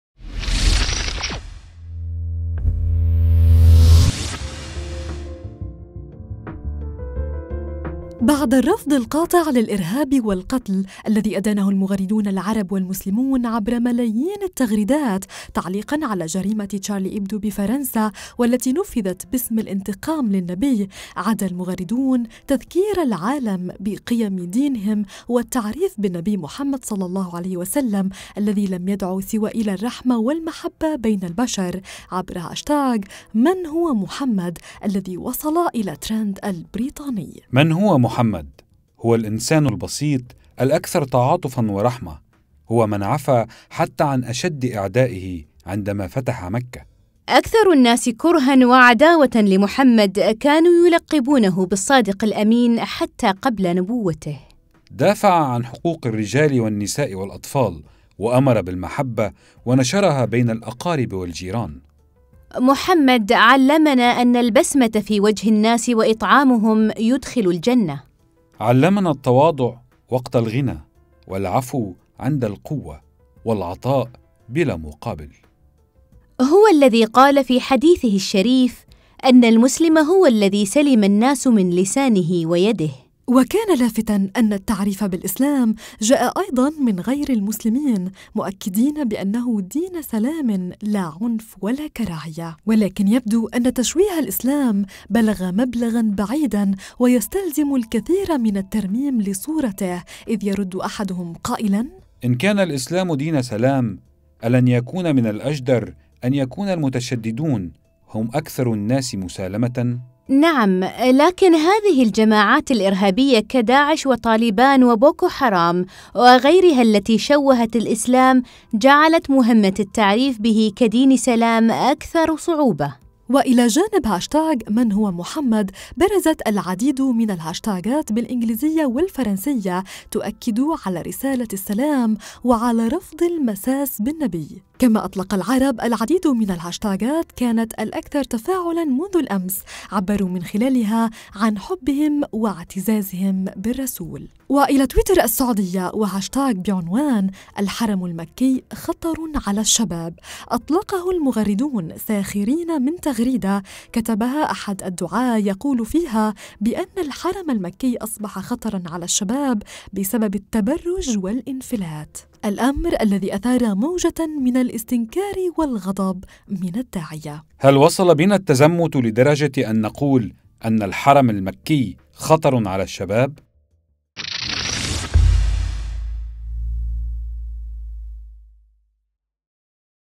نشرة تويتر (351): من هو "النبي محمد".. حوار المغردين حول العالم
بعد الجدل الكبير الذي أثارته العمليات الإرهابية الأخيرة في فرنسا باسم الإسلام، أطلق المغردون العرب حملة عبر تويتر، للتعريف بالإسلام والنبي محمد (ص)، والتأكيد بأن الإسلام دين سلام، عبر هاشتاق إنكليزي "من هو محمد"، كيف تفاعل عبر هذا الهاشتاق المسلمون وغير المسلمين؟ وفي النشرة أيضاً جدل واستنكار عبر هاشتاق "الحرم المكي خطر على الشباب"!